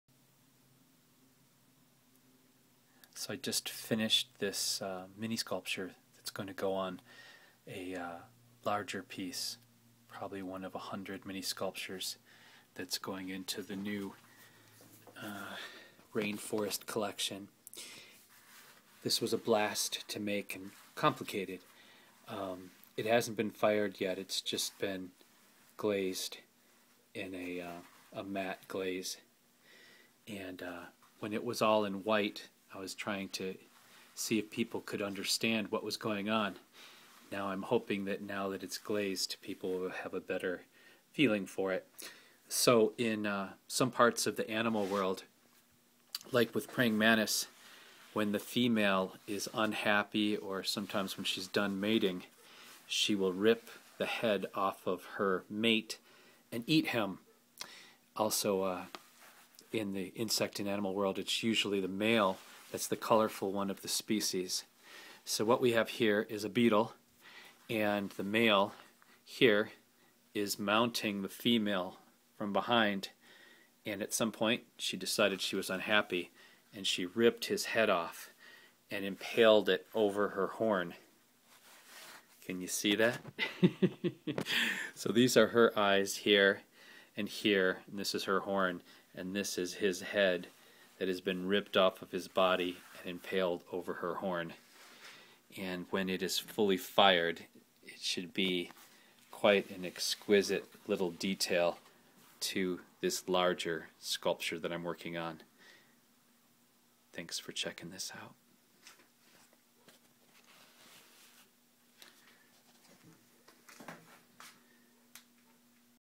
Beetle sculpture
Pre fired . Sculpture of beetles mating. By the artist Scott Riddle.